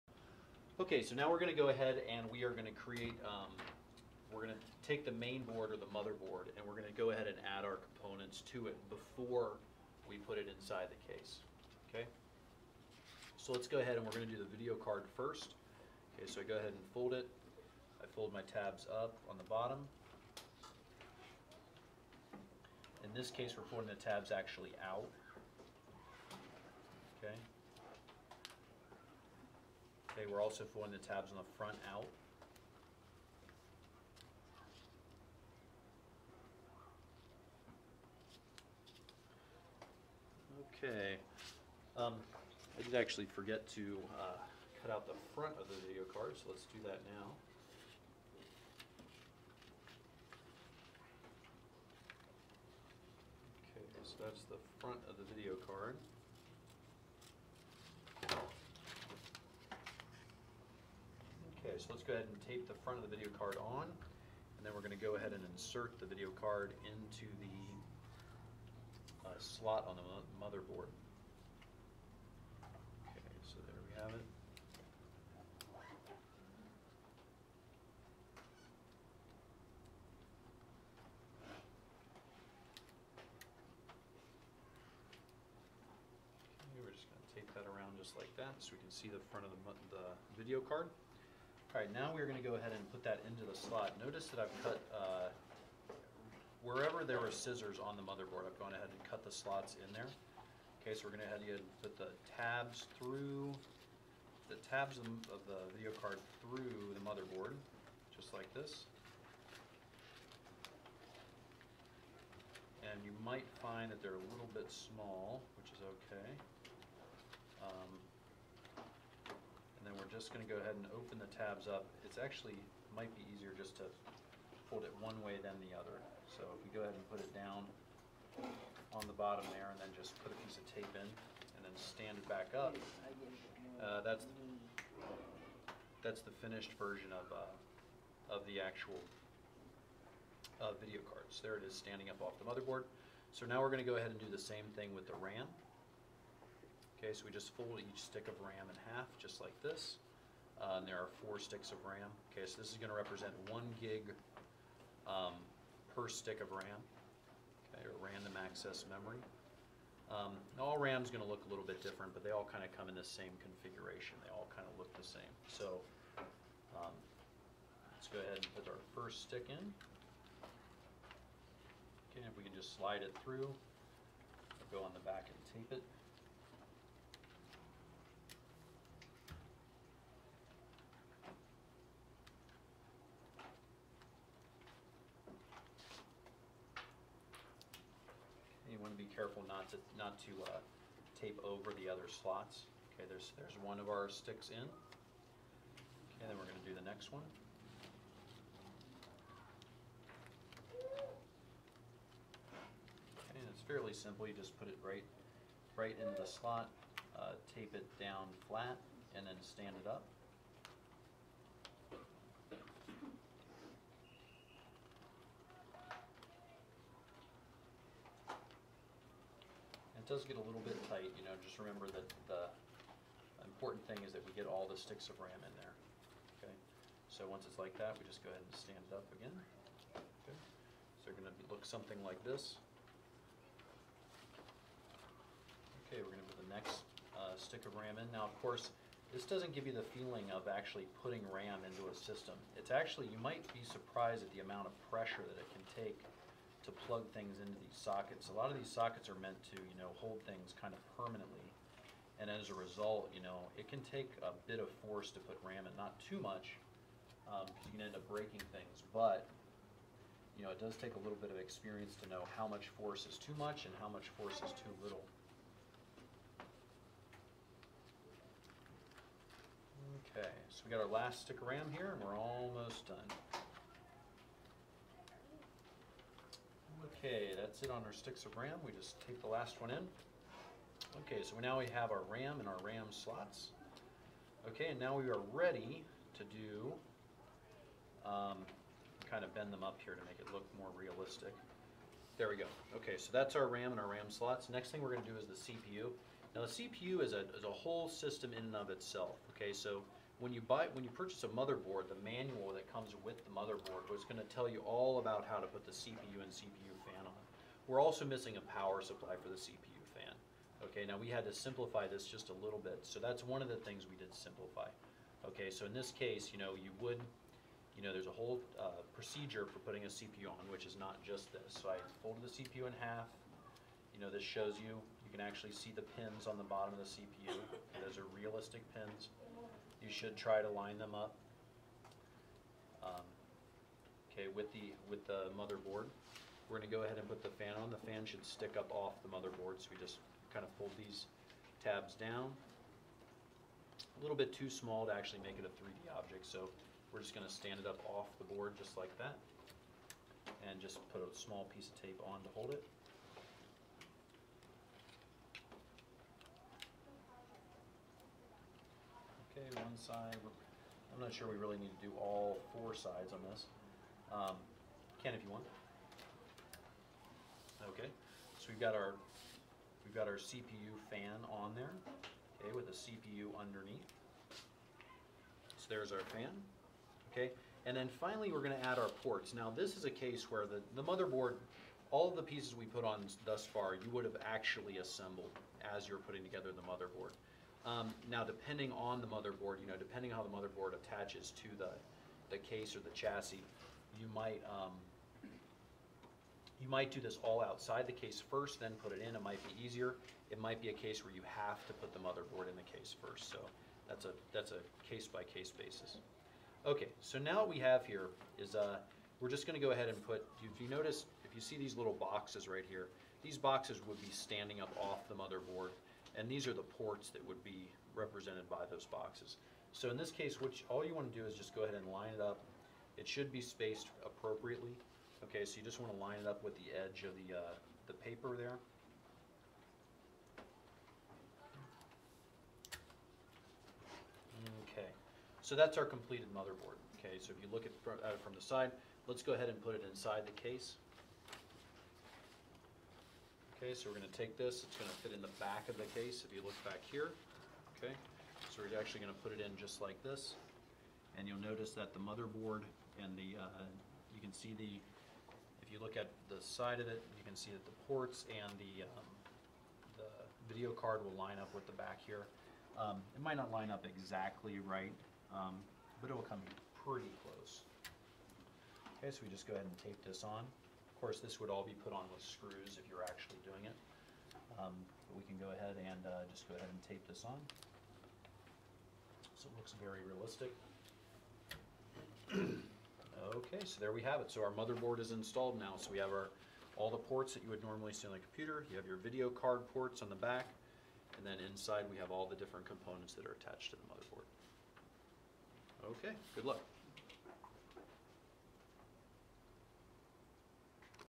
Paper Computer Project : Step 3 : Assemble and Install Main Board and Components
Assemble and install the Main Board (Mother Board), RAM, CPU,CPU Fan, and Video Card . Instructions on assembling the PaperComputerProject.com paper model P.C. A lesson from PaperComputerProject.com for children of all ages. Build your  first P.C. out of paper on your way to building the real thing.